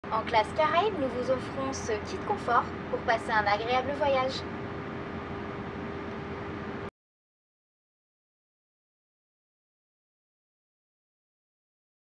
Le kit confort Caraïbes, fort en petits plus !!
En classe Caraïbes, nous vous offrons une trousse confort bi-matière d'inspiration végétale en coton recyclé et toile de jute réutilisable, contenant les éléments suivants :
Une gamme de 3 masques de sommeil pour se reposer à collectionner sur nos vols
Des écouteurs
Des bouchons d’oreilles en mousse à mémoire de forme
Une brosse à dents et son tube dentifrice
Une paire de chaussettes